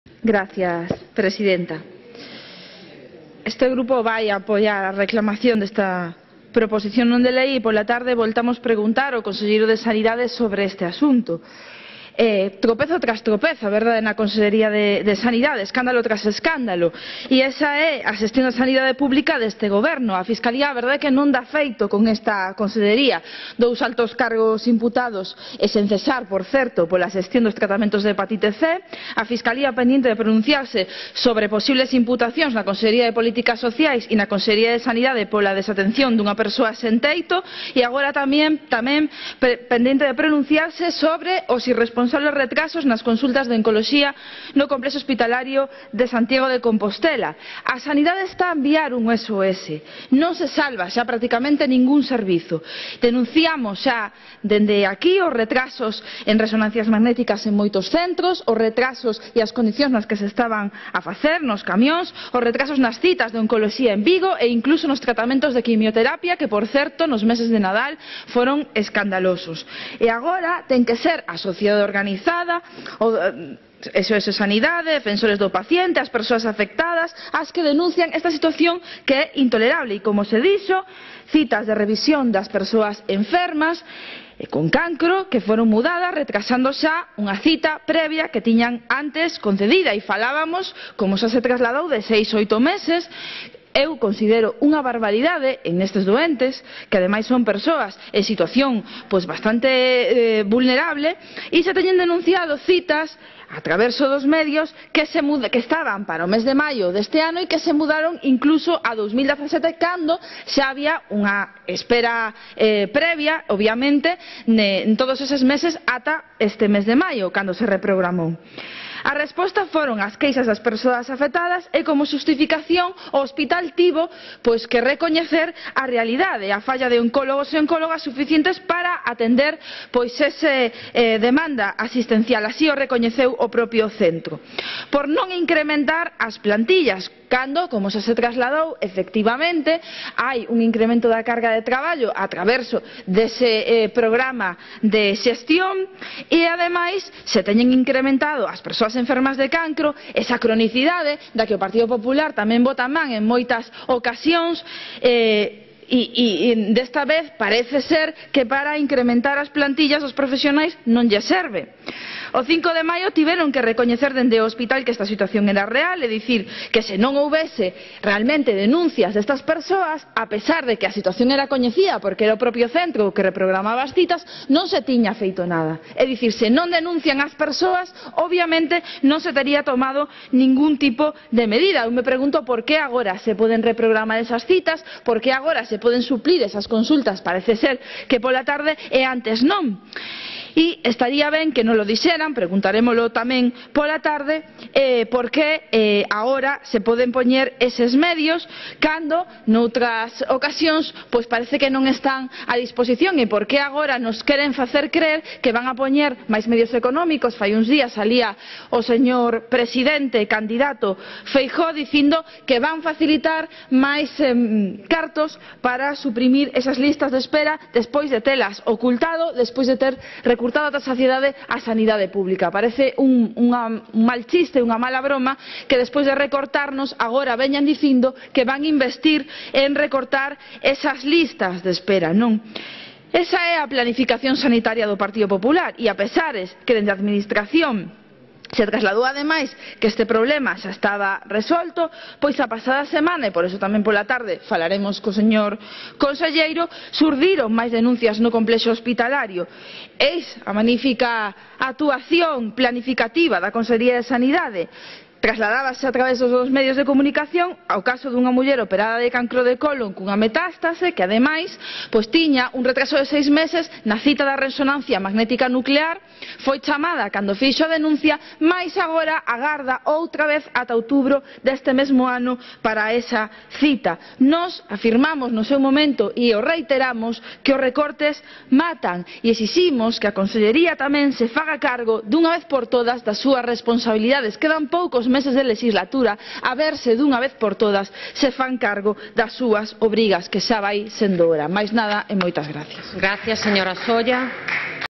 "A sanidade pública galega está a enviar un SOS e non se salva ningún servizo"
A viceportavoz de AGE, Eva Solla, denunciou a intolerable situación na que se atopa a sanidade pública no noso país a consecuencia dos continuos ataques do Goberno galego do PP durante estes anos con continuos recortes. Agora o "presidente-candidato Núñez Feijóo" atrévese a prometer que se investirán máis cartos en solucionar os retrasos de ata 8 meses das citas a doentes de cancro recoñecidas polo propio hospital de Santiago.